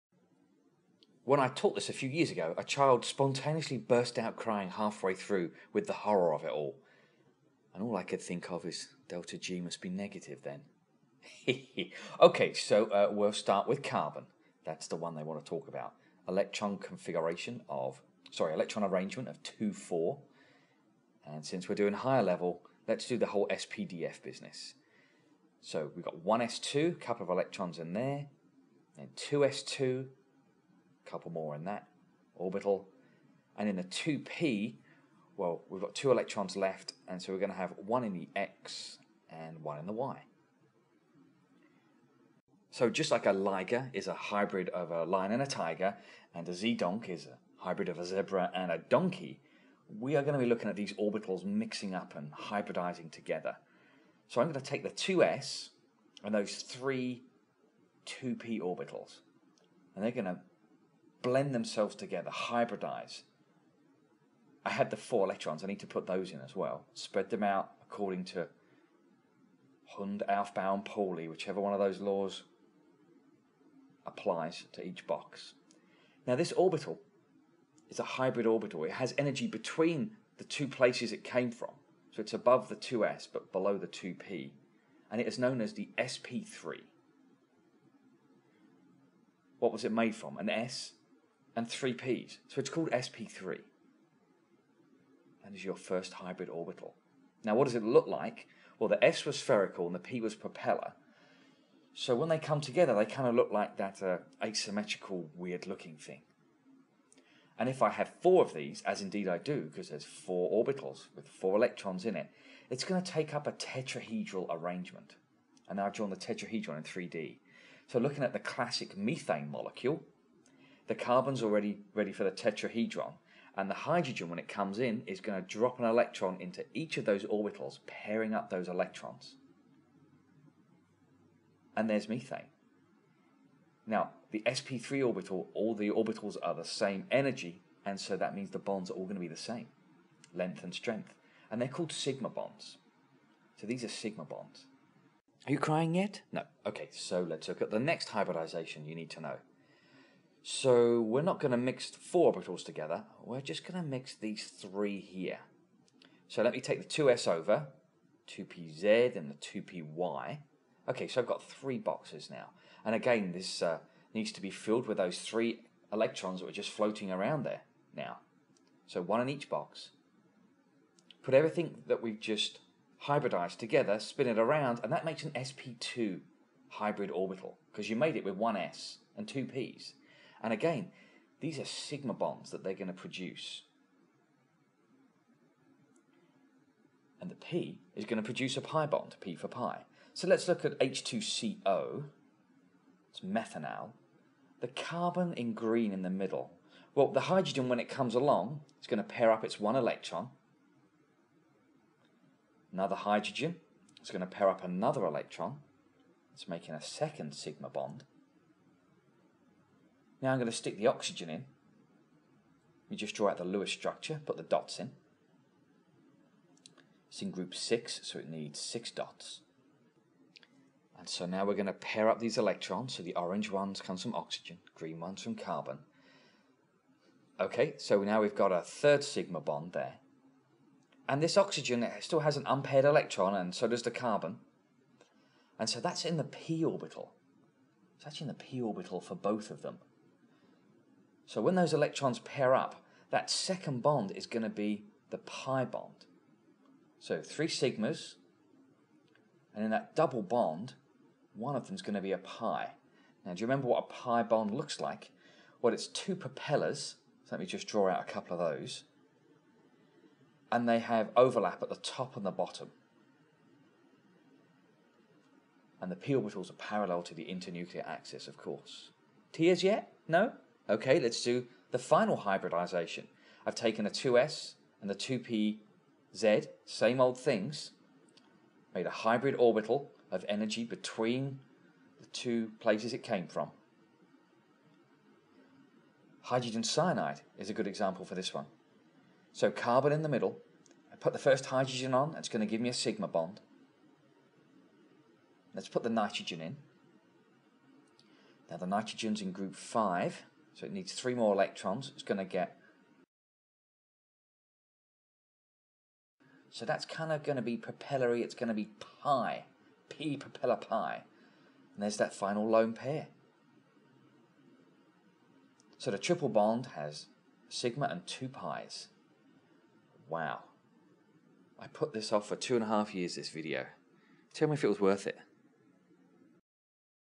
S2.2.15 Explain hybridization as mixing of orbitals making new orbitals [HL IB Chemistry]
Minor error at 4:27: pi bonds are PERPENDICULAR (not parallel) to the internuclear axis.
 Nasty business this.  Once or twice the IB asked a question that needed the details presented here -- but more often than not a simple treatment is required.